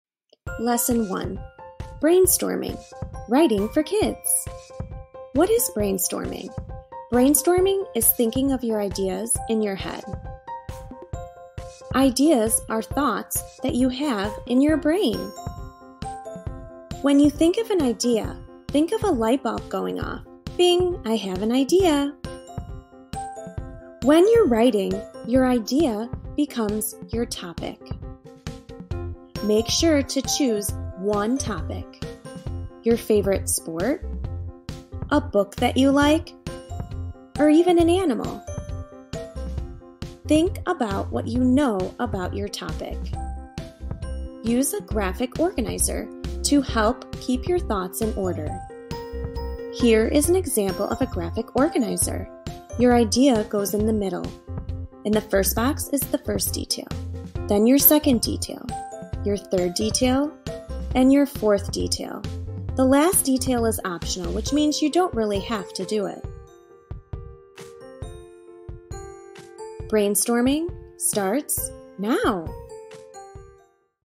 Lesson 1: Brainstorming
This video describes what brainstorming is and introduces how to use a graphic organizer.